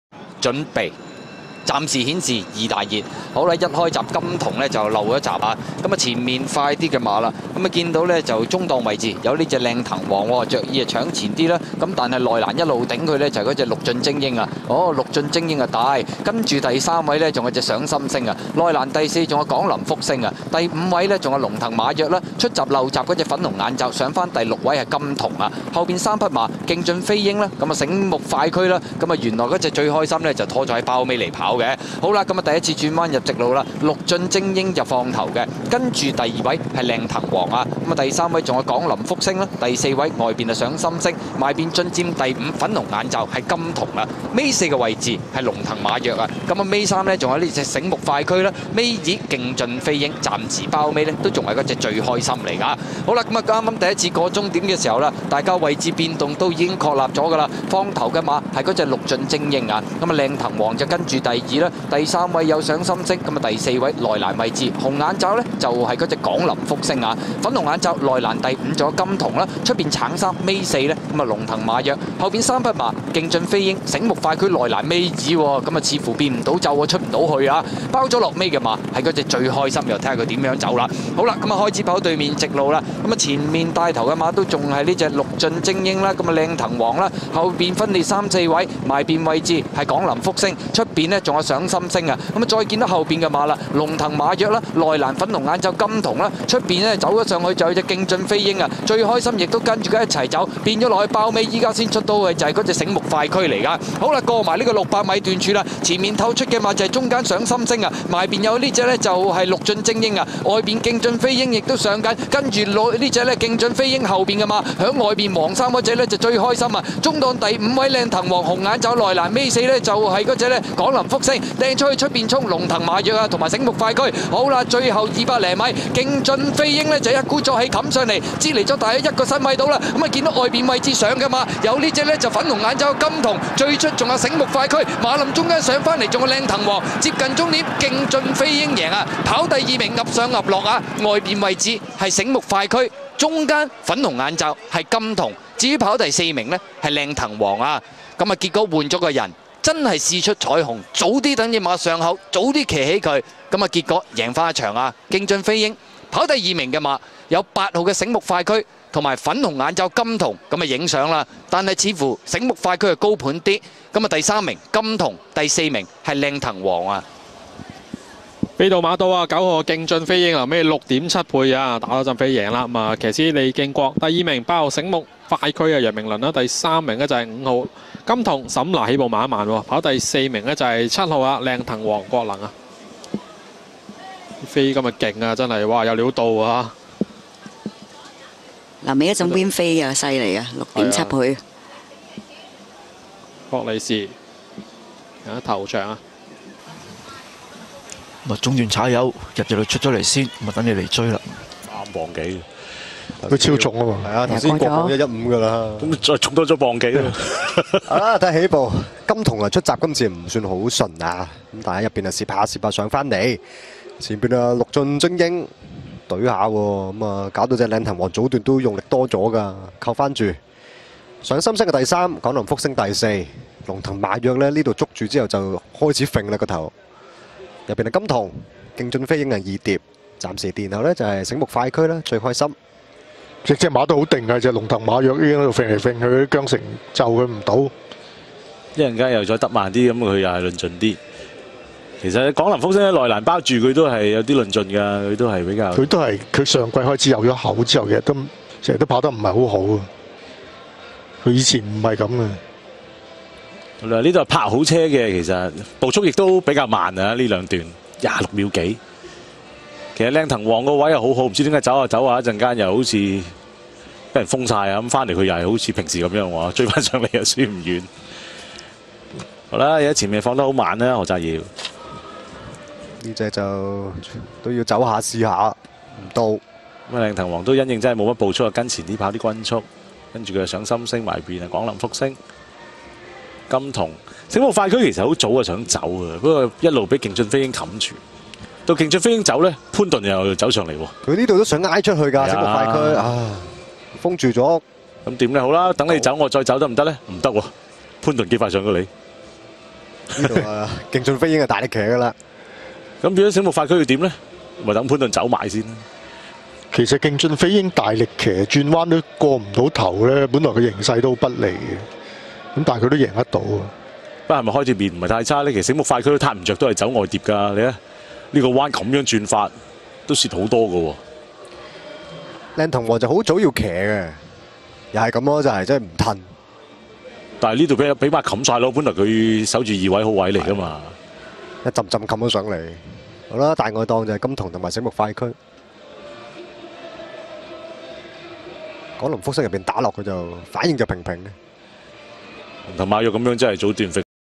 2019/20 (133) - Young Glory (競駿飛鷹) - 利敬國
Jockey : G van Niekerk (利敬國)
Trainer : DE Ferraris (霍利時)
Owner : HKJC Racing Club Limited (競駿會有限公司)

2019/10/30 - Hong Kong - Happy Valley (133)